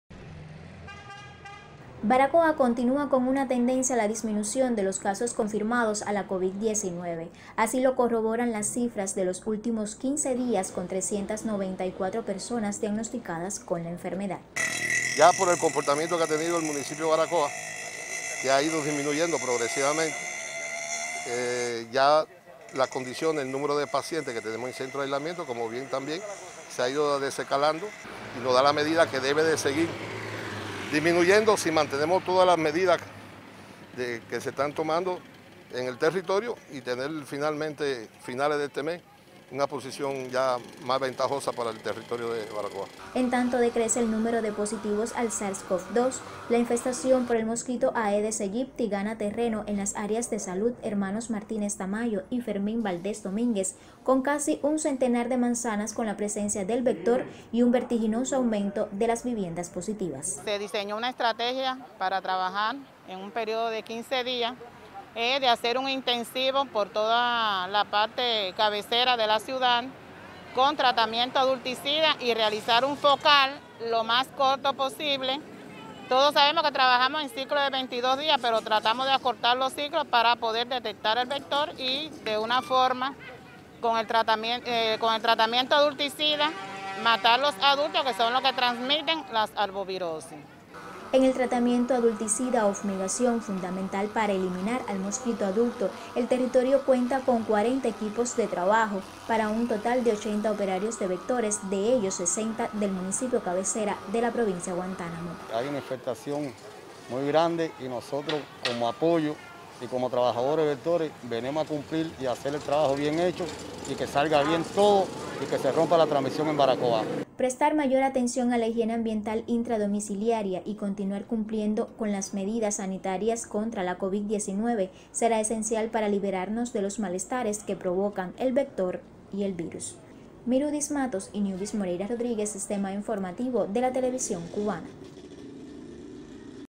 Baracoa por estabilizar situación epidemiológica
Pese a que el número de casos positivos a la Covid-19 decrece en Baracoa, la situación epidemiológica continúa tensa, ya que aumenta la infestación por el el mosquito aedes aegypti.